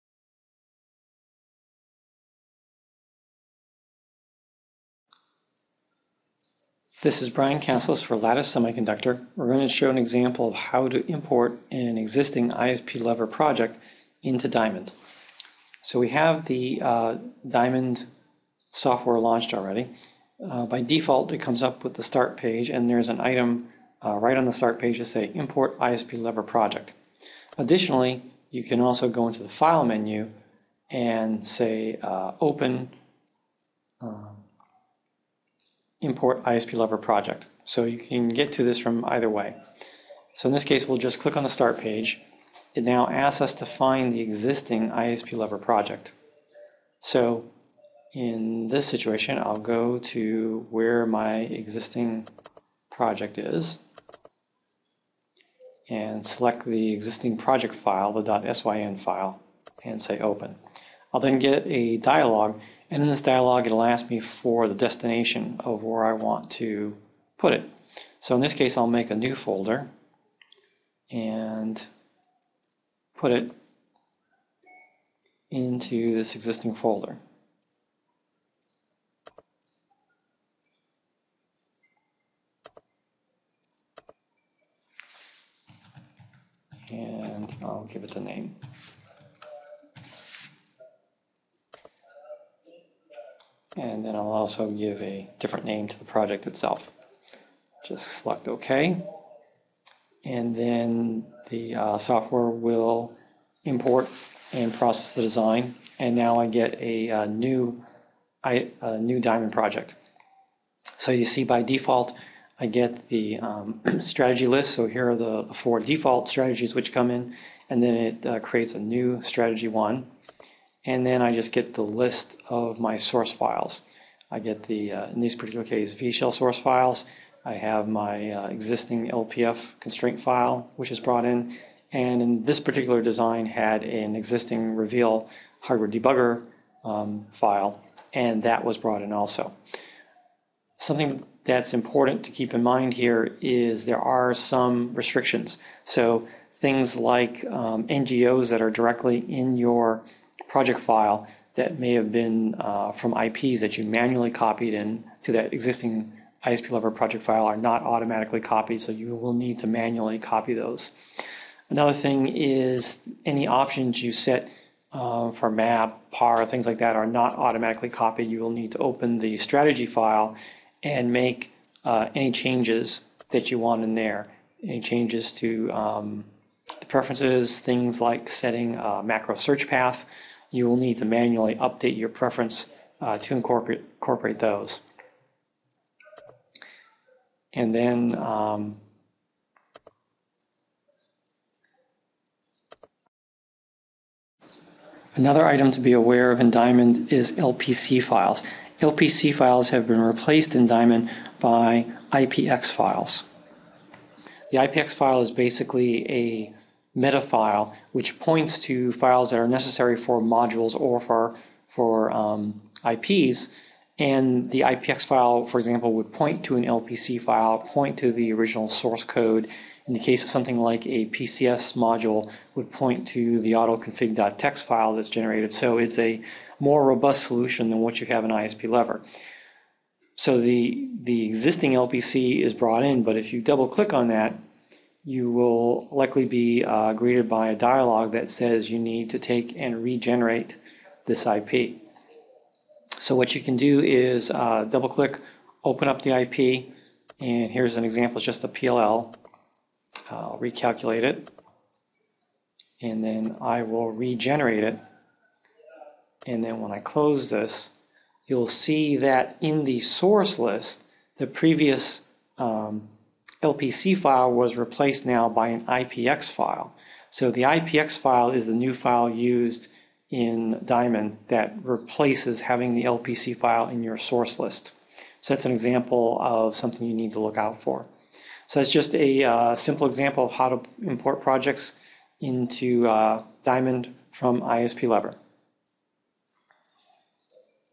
Diamond Importing from ispLEVER
Lattice Diamond software uses a different project structure than the previous ispLEVER software. This video describes how to import an ispLEVER project into Diamond.